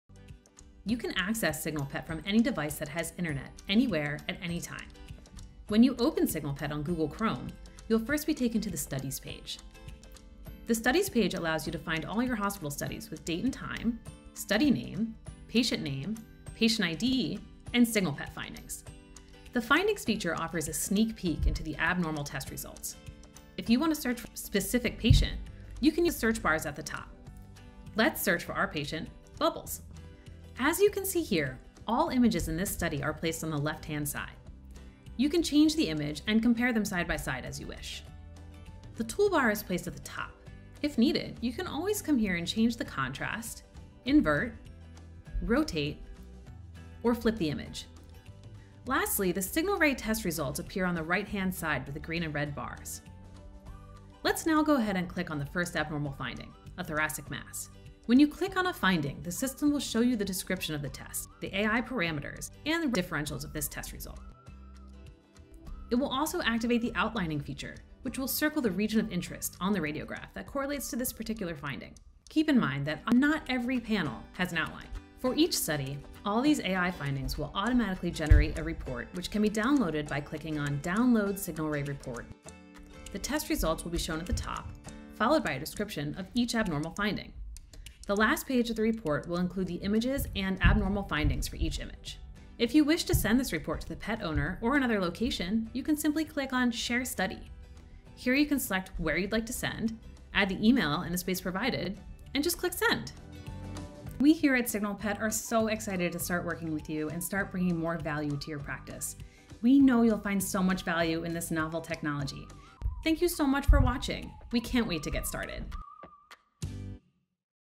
SignalPET User Tutorial
Get to know SignalPET's advanced veterinary radiology AI technology with this helpful user tutorial. Watch this video to quickly learn how to navigate SignalPET's software and always know how to find what you are looking for.